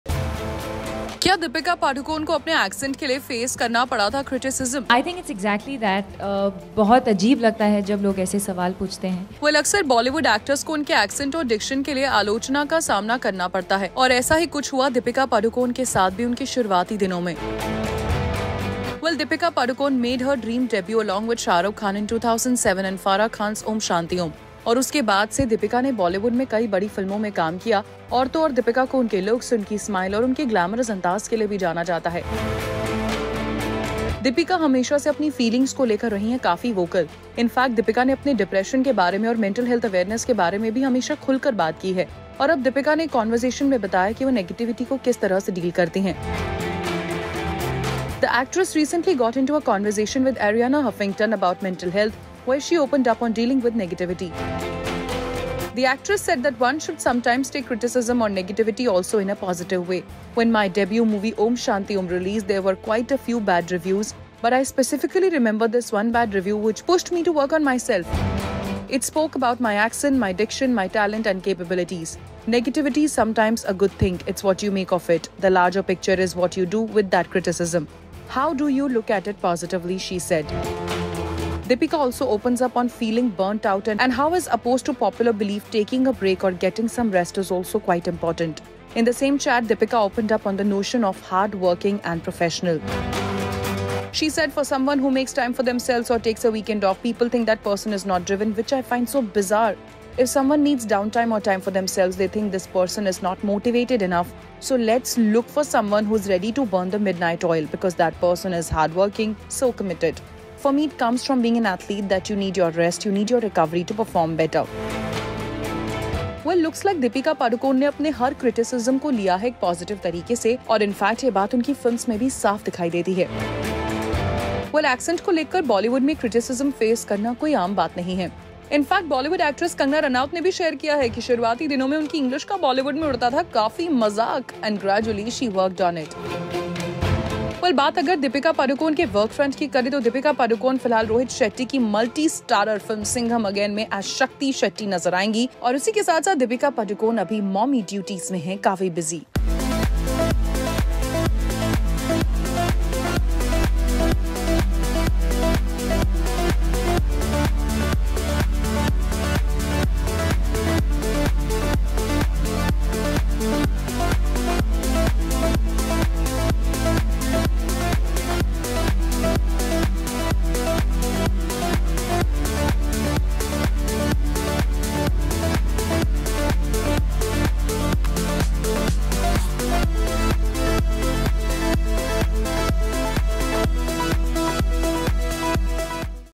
Deepika Padukone Opens Up on criticism for her accent in Om Shanti Om,' It pushed me..'
Deepika Padukone talked about facing criticism for her role in the 2007 film Om Shanti Om. Some viewers questioned her talent and mocked her accent and diction, even though her voice was dubbed by someone else in the movie. In a conversation, she shared how she dealt with the negativity during her debut film. This comes as she faces trolling for her dialogue delivery in the trailer of Singham Again, where she plays a police officer.